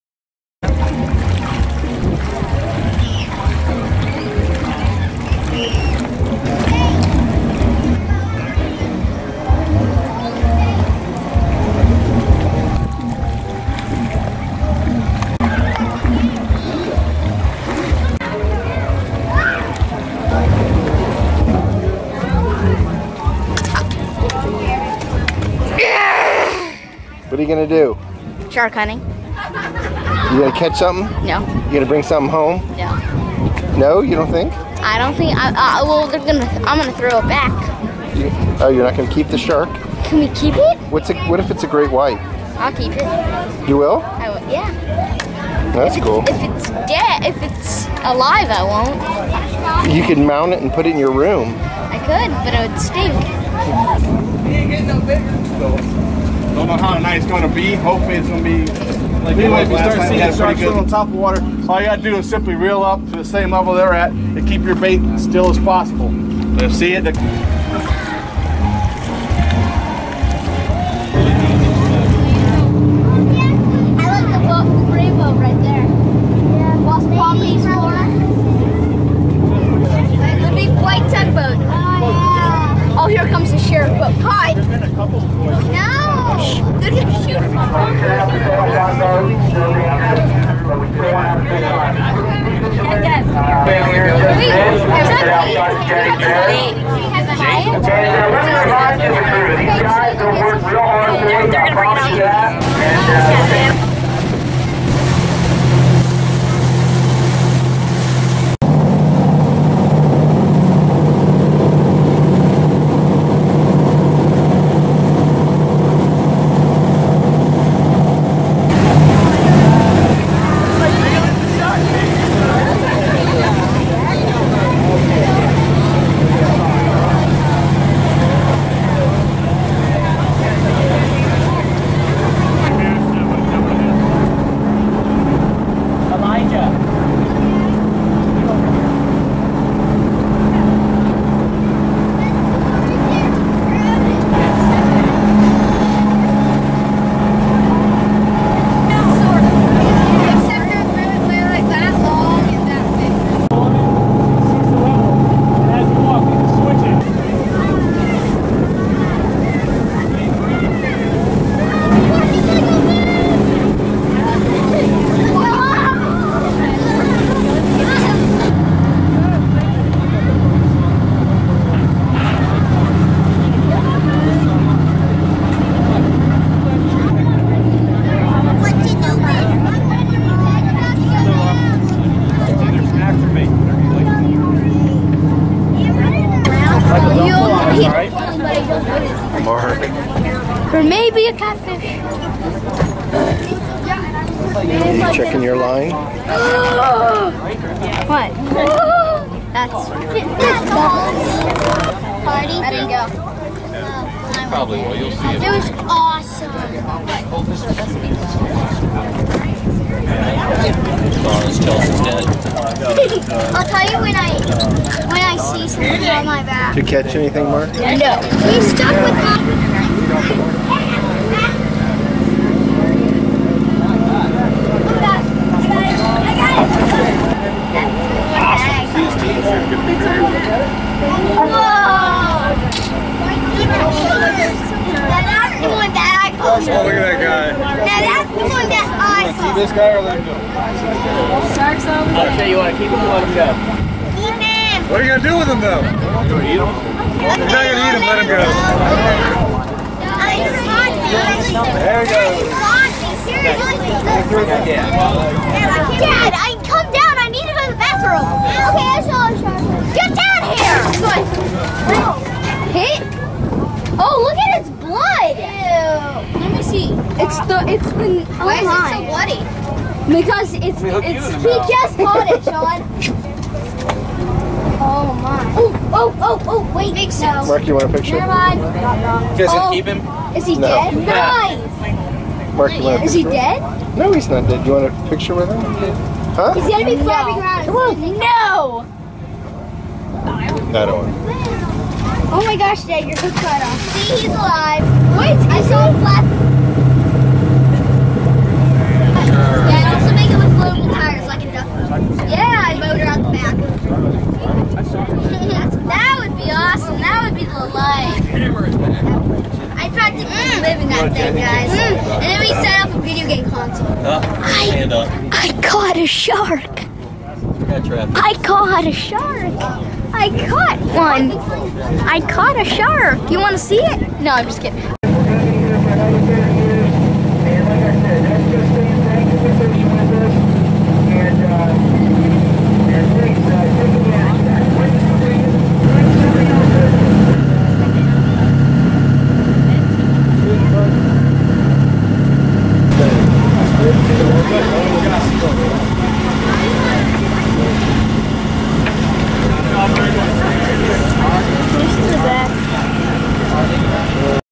Shark Fishing at Port Canaveral
Nighttime ocean shark fishing aboard the Miss Cape Canaveral on Sept. 1, 2012 in Port Canaveral, Fla.